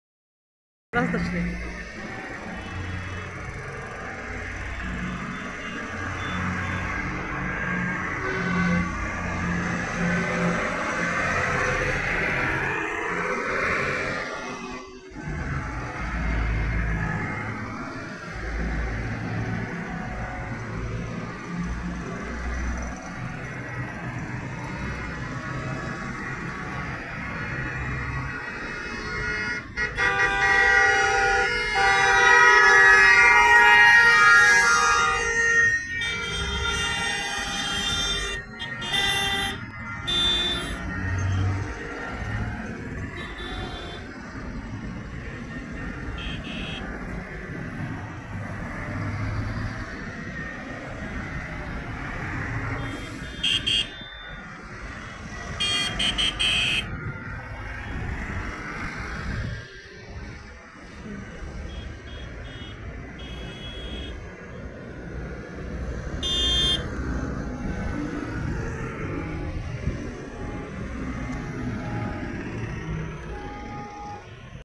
Фермерские протесты 8.01.2024
И мимо нас прокатилась часть тракторов.

And some of the tractors rolled past us.

Und einige der Traktoren rollten an uns vorbei.